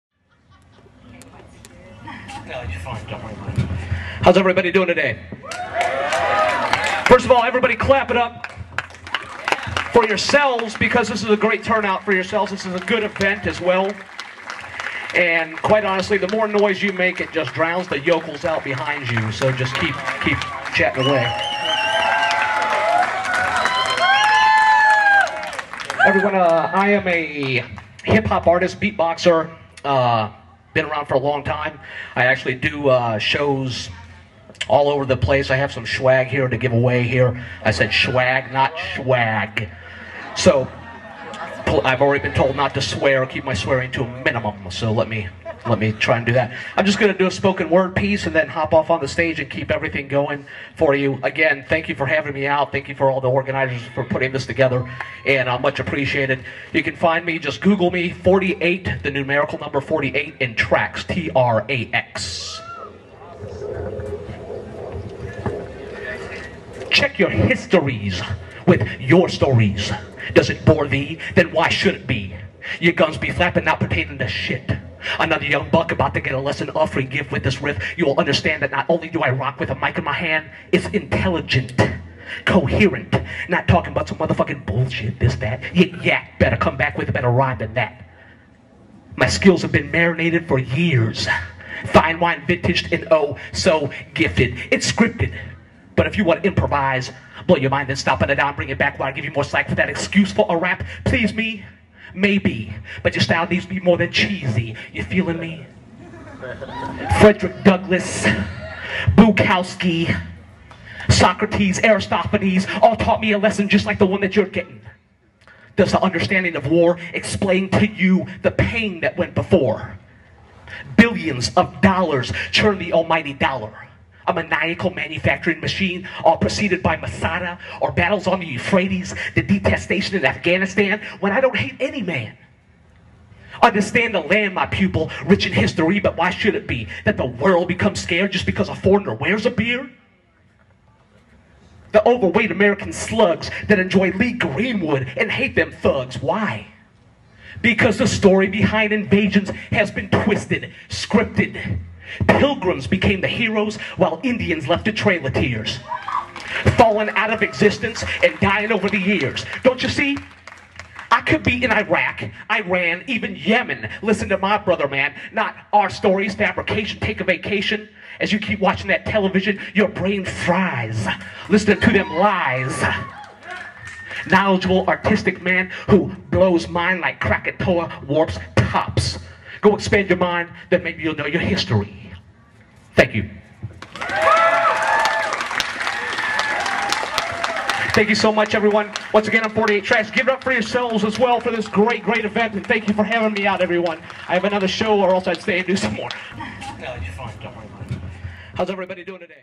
48 Trax Performs at Peace March
On 9/11/2008 the Denton Texas chapter of Students for a Democratic Society organized a peaceful protest of over two hundred citizens to protest the illegal wars in Afghanistan and Iraq, demand an independent reinvestigation of the events of september eleventh, and truth in military recruiting on campus.

The protest consisted of students, teachers, veterans, business owners, politicians, artists, musicians, and even whole families that came to show support.

Speakers at the event included:
Beverly Davenport, asst. Professor of Anthropology at the University of North Texas

Keith Brown, Philosopher and faculty member of the University of North Texas Philosophy Department

Lon Burnham, Director of the Dallas Peace Center and Texas State Representative

John Rhodes, local business owner and tattoo artist

And student members of SDS, CEDP, and more.

Bands that played at the event included:
Singer/Songwriter Delphi
Freddy of the Heart String Stranglers
Hip-Hop artist 48 Trax
and Dirty Hairy